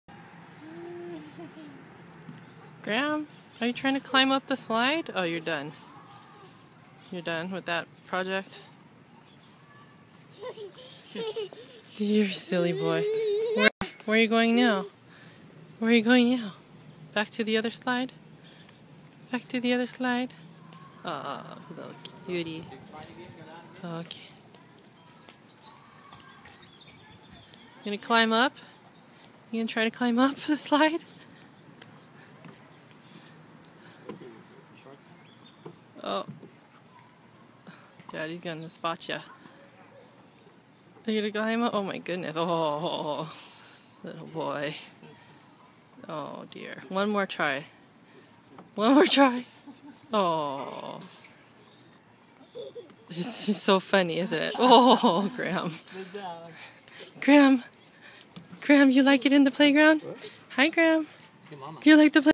Graham at Playground (part 2)
He's figuring out the slide!